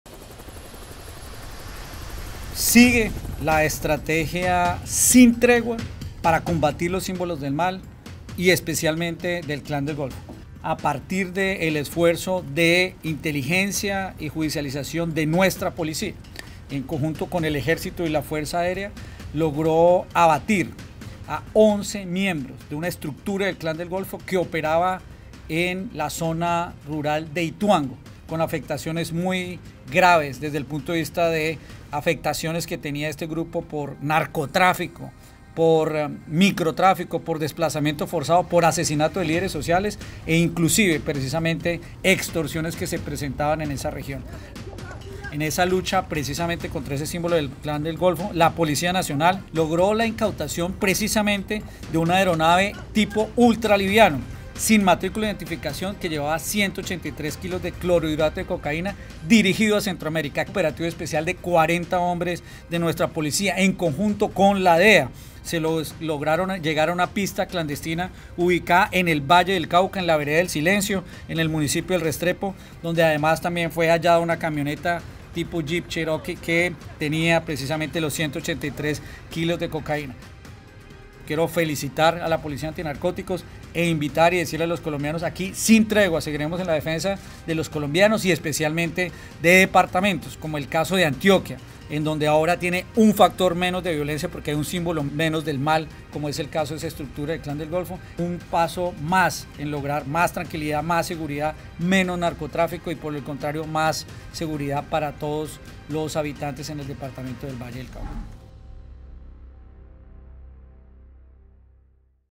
Golpe contundente contra el Clan del Golfo | 04.02.2022
Inteligencia de las Fuerzas Militares lograron un golpe contundente contra el Clan del Golfo en el Valle del Cauca, 11 miembros neutralizados , incautación de aeronave ultraliviana con 183kg de cocaína en su interior.

Seguimos combatiendo los Símbolos del Mal